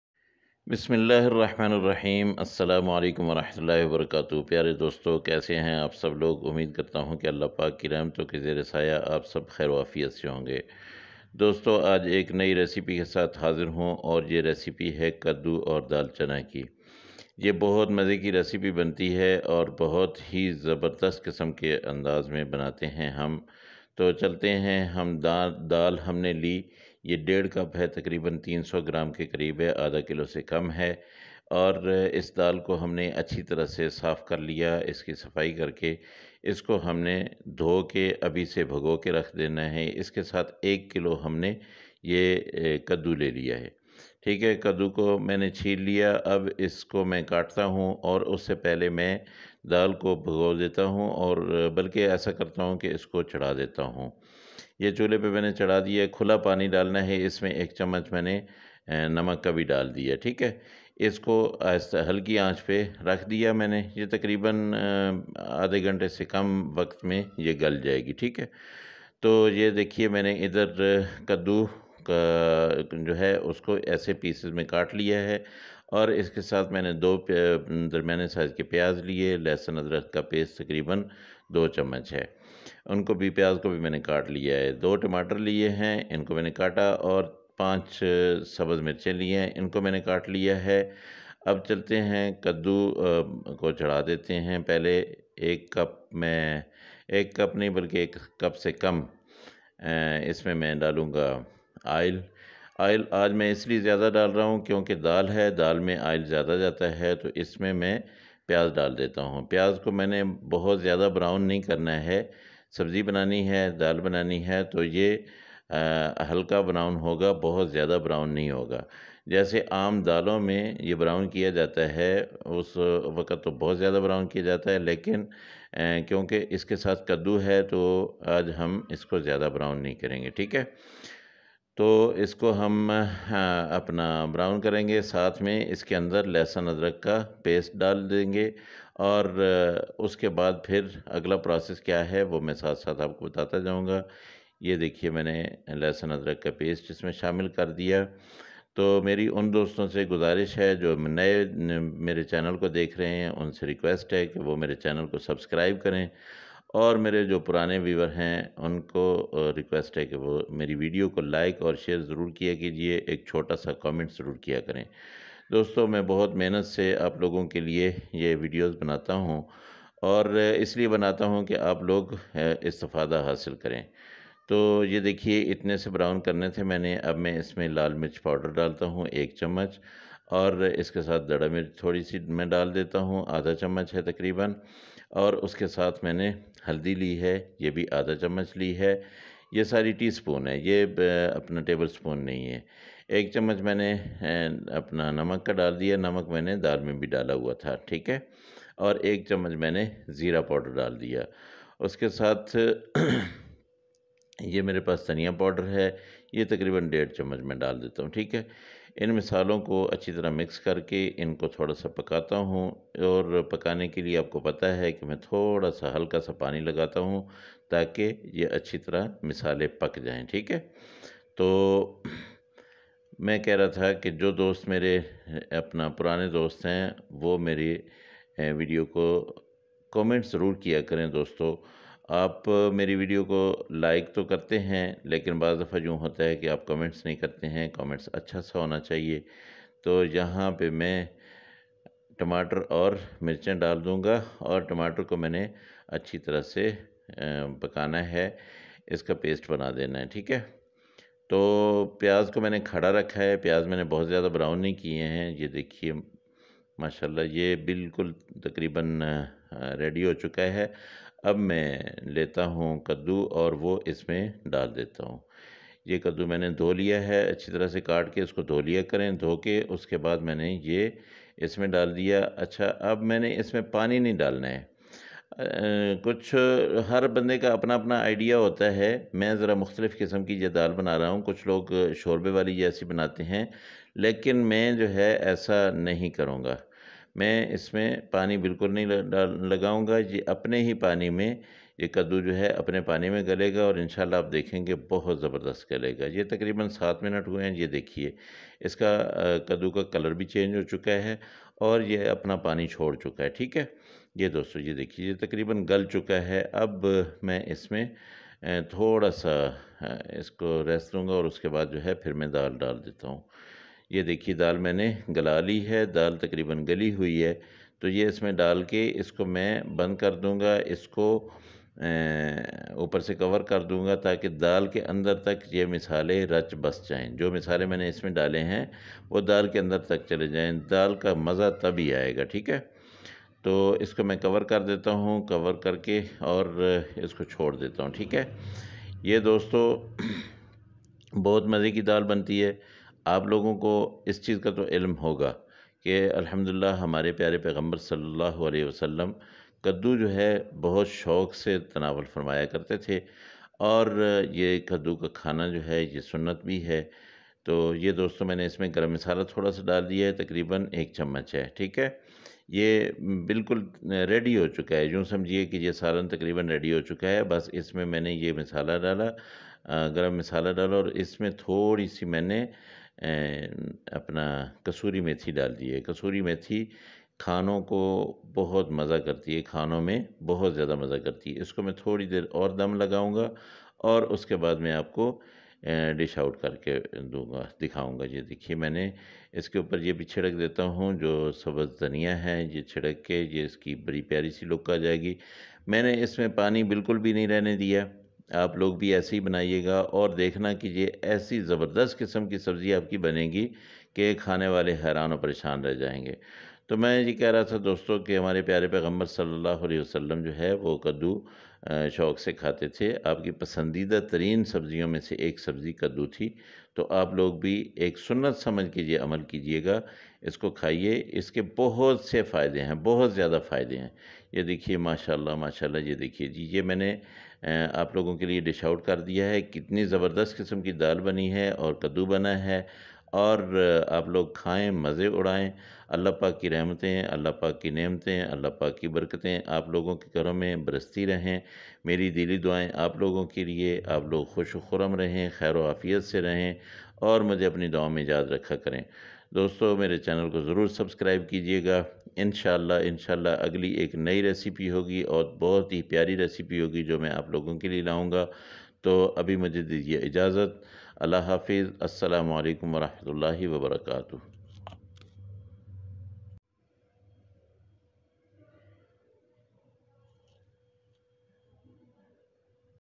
Kaddu Daal Chana || Special Veg Recipe || کدو دال چنا بنانے کی ترکیب
loky daal chana bananay ka sahi aur aasan tareeqa
cook shok food